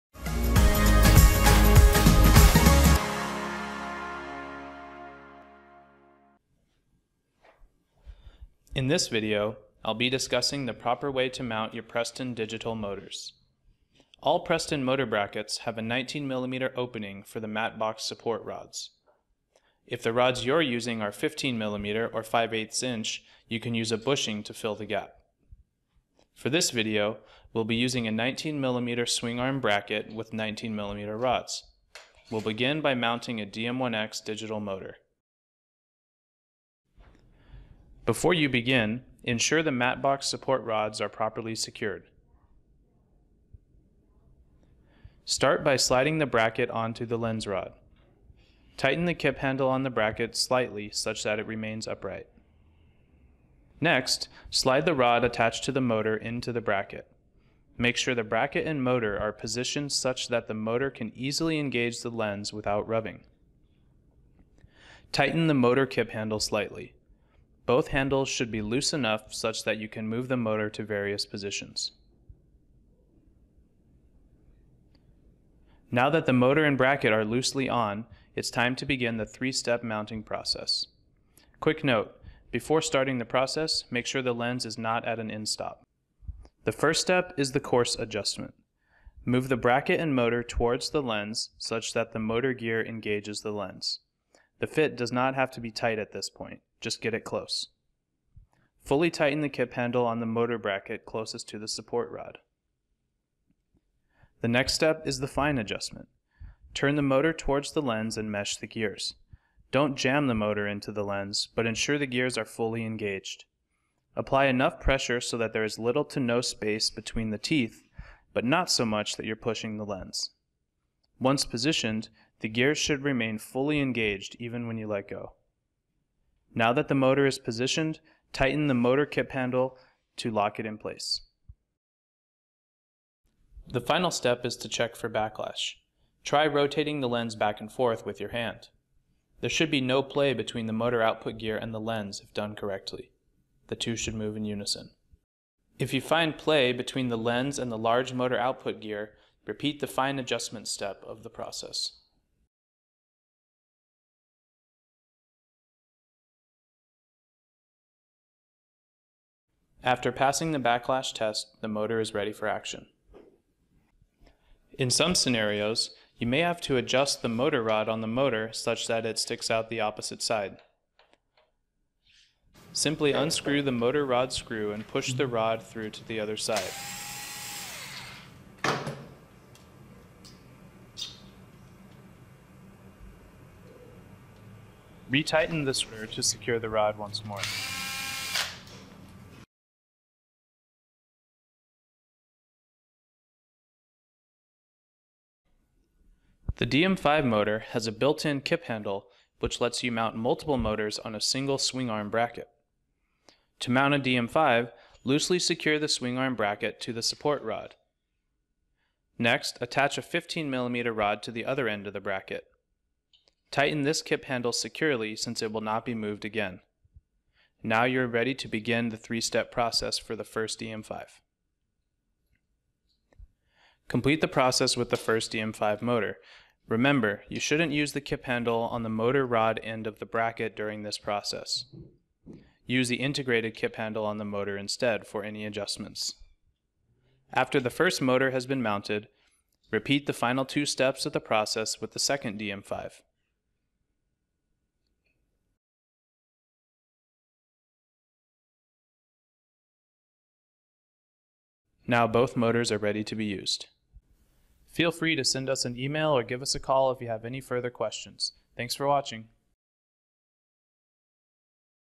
PCS: How to Mount A Motor
In this video, we'll show you the proper technique for mounting a Preston motor using a DM1X, DM5 and a 19mm swing arm bracket.